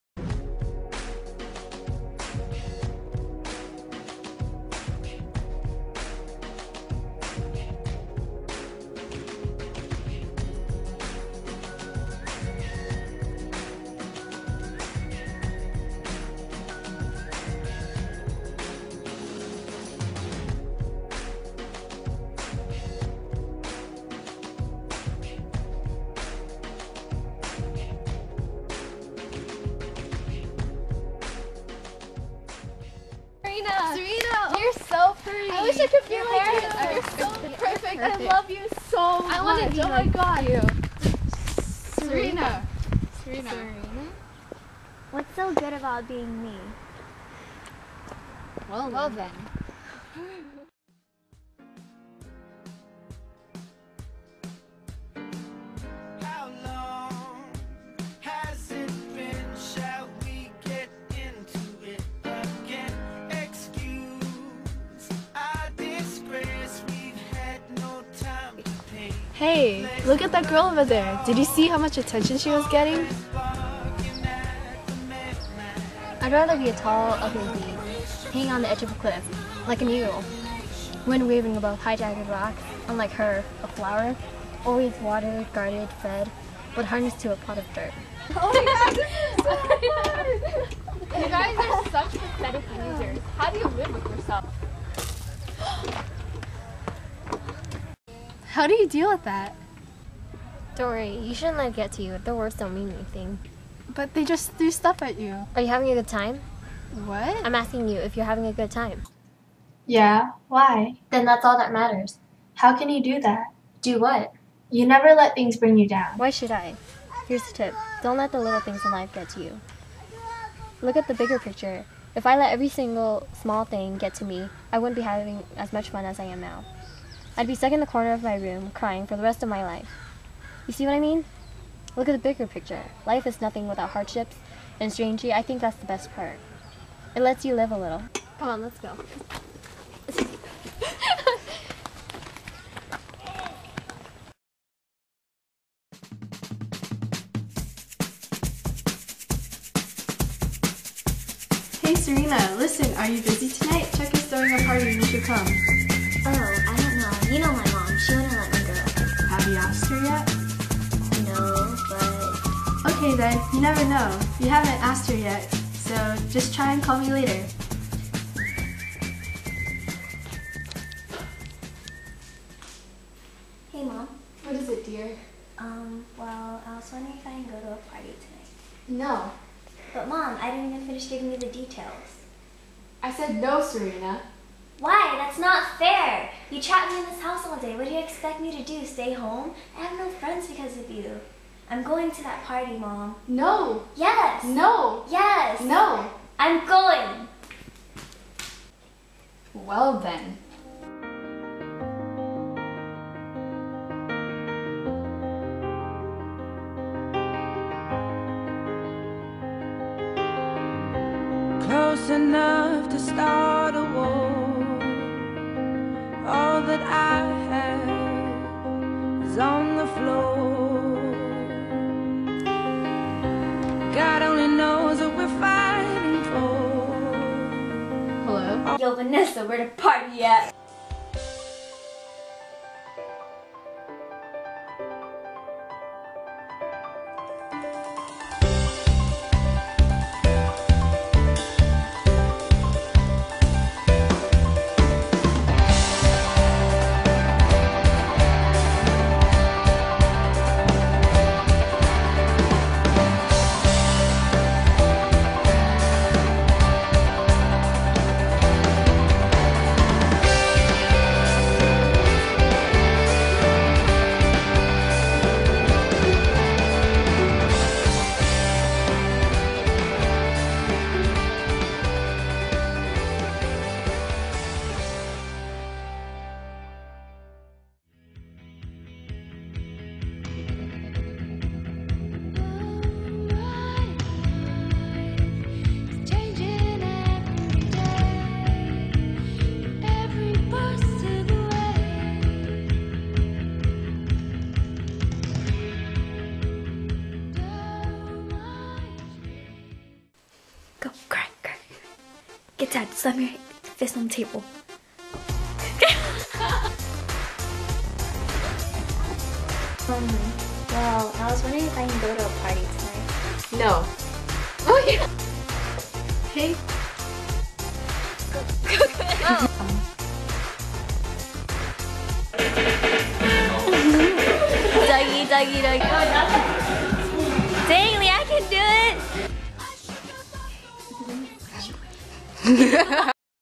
IDENTITY
Our English project that we spent 14 consecutive hours working on based off of the poem "Identity" by Julio Polanco! This looks more like a romance movie though.. \:

Lyanh Vang
Stephanie Bui
Katherine Huynh
Jade Truong